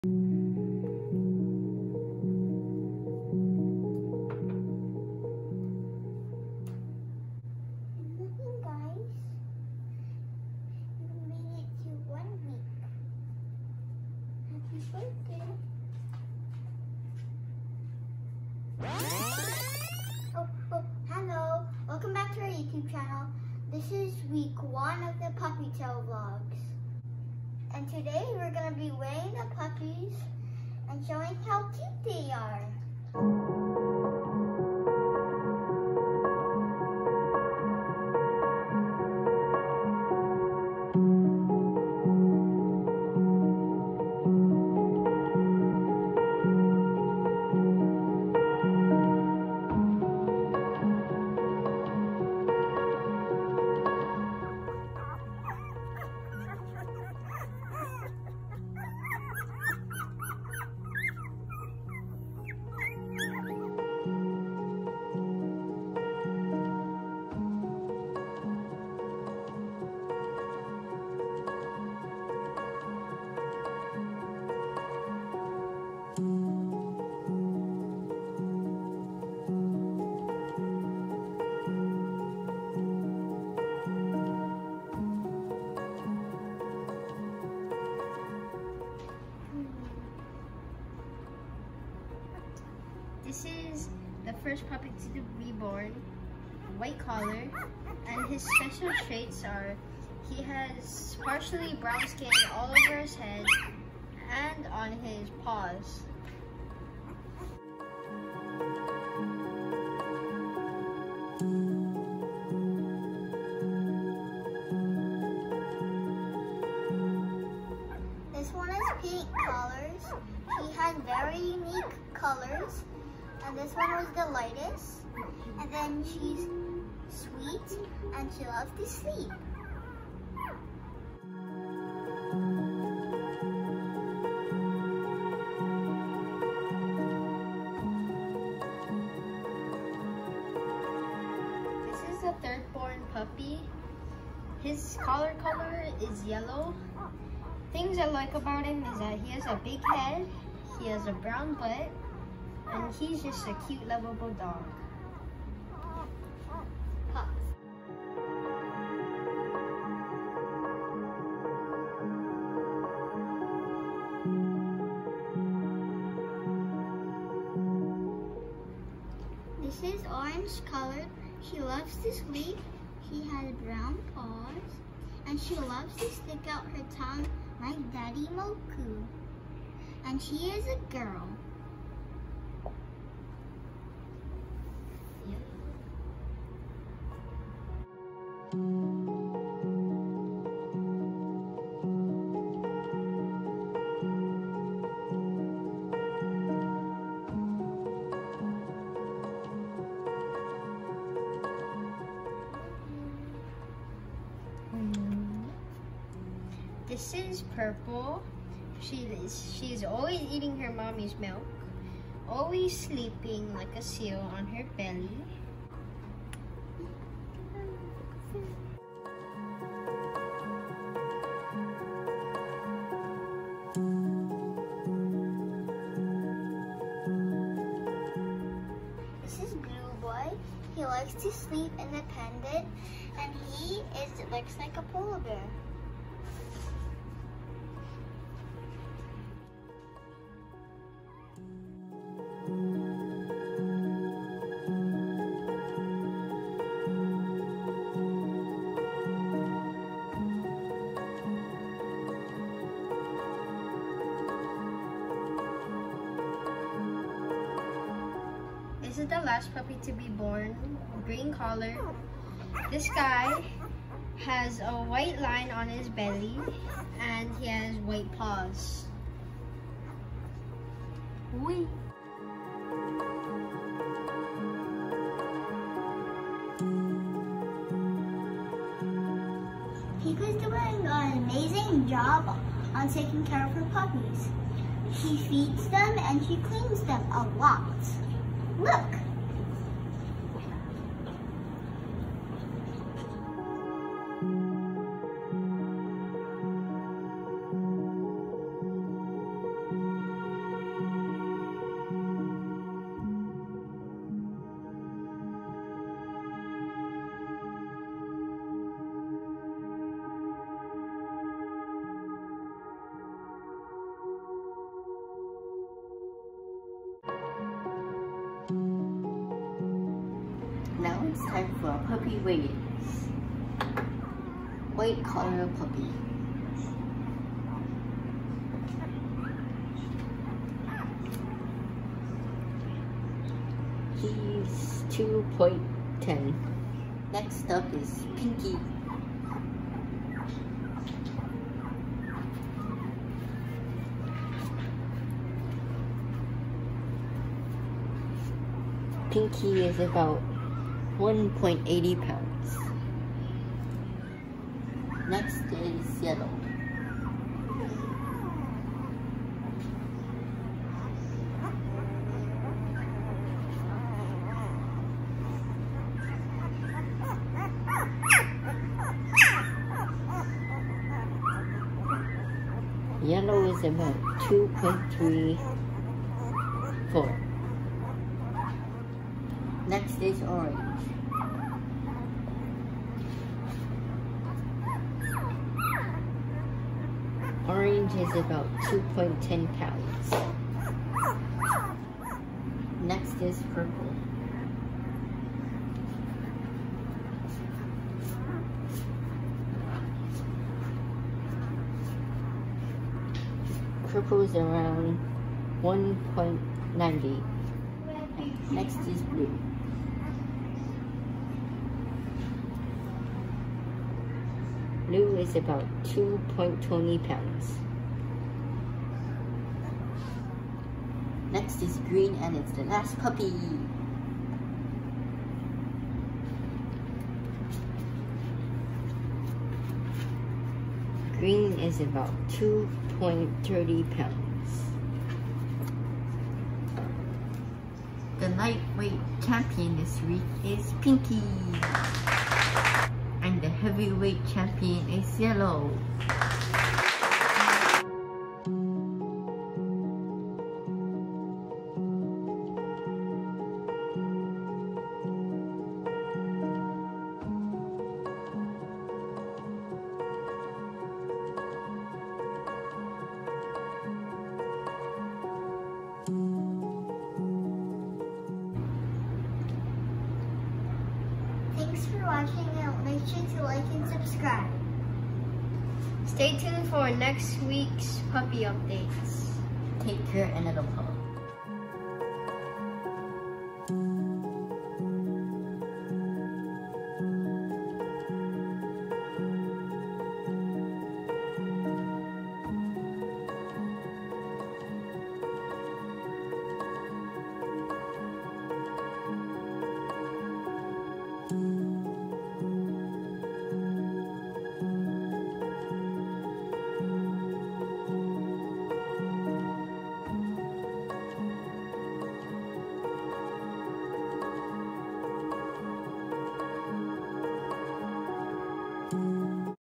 Puppy Tail Vlog | Week 1 | Golden Retriever & Akita puppies
The puppies are one week old! They are doing great, just like mama Pika!